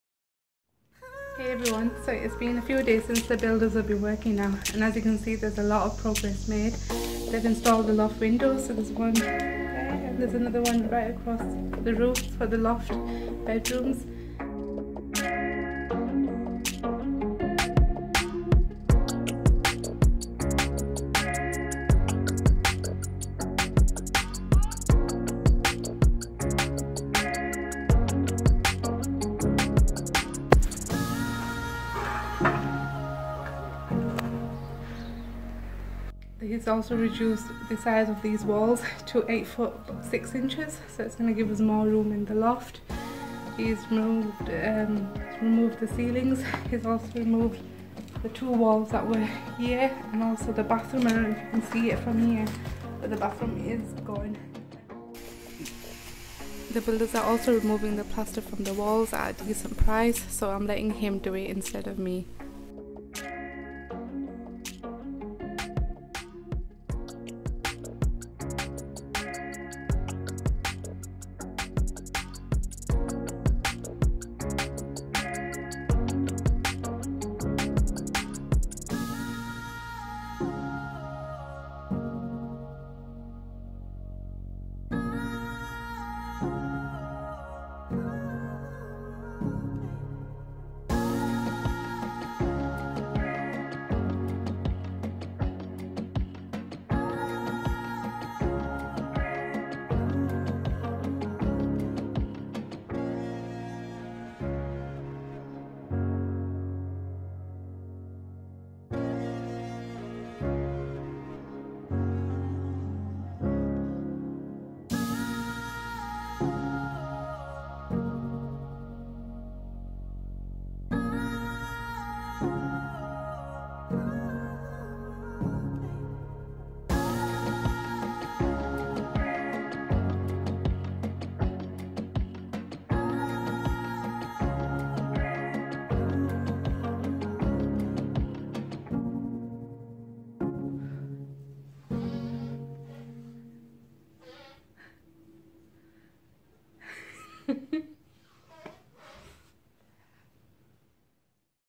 Easy DIY Home Renovation - Removing the ceiling and walls
update on how much work the builders have recently done. the ceilings and walls have been removed back to brick. video shows a quick update on progress made of the construction